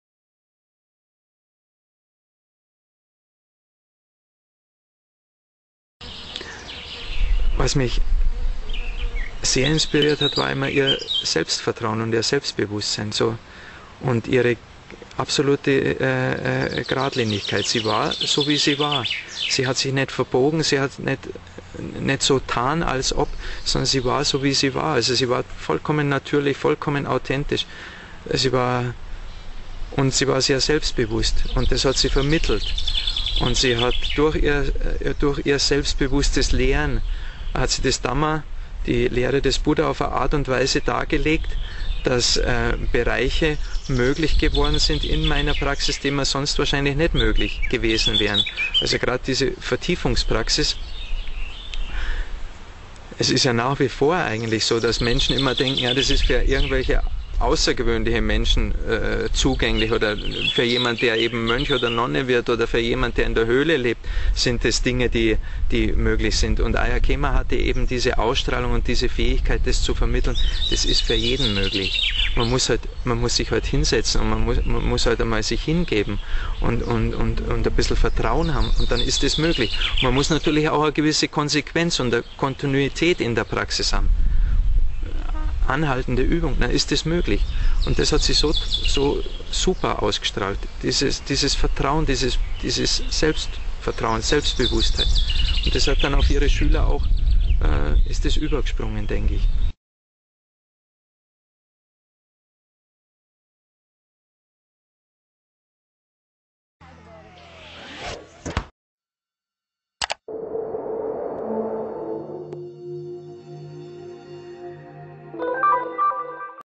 Bhante Nyanabodhi (18) Inspiration bei Ayya Khema
Bhante Nyanabodhi erzaehlt, was ihn an Ayya Khema am meisten inspiriert hat - zum Selbstvertrauen